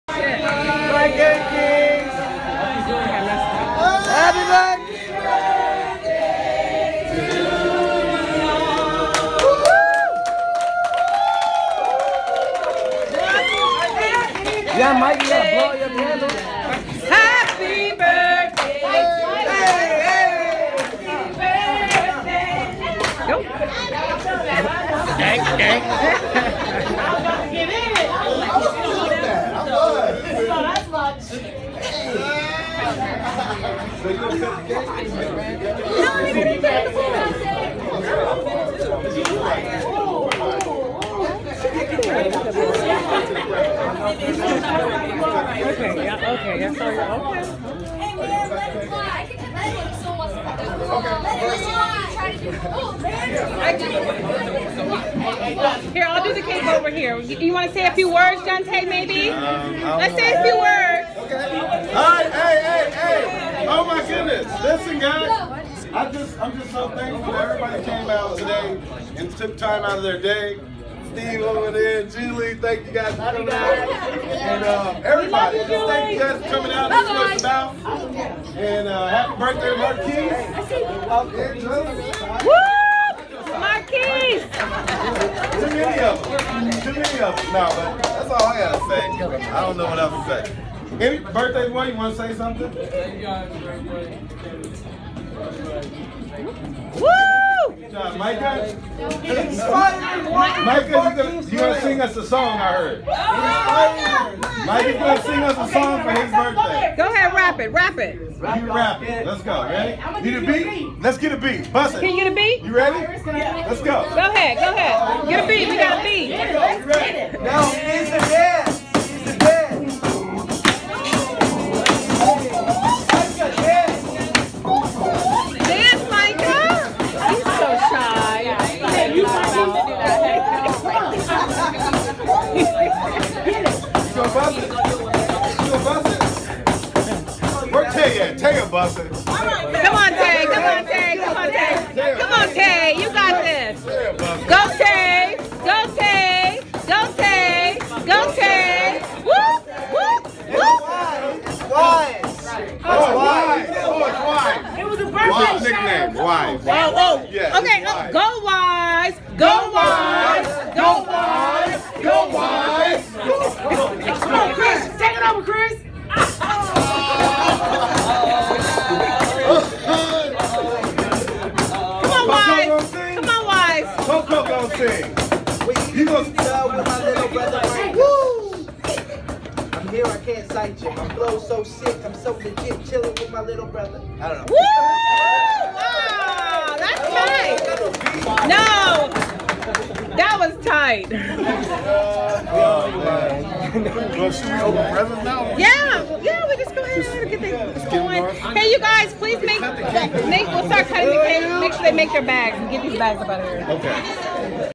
Micah and Marquece birthday 2016
Quece turns 19

Micah turns 11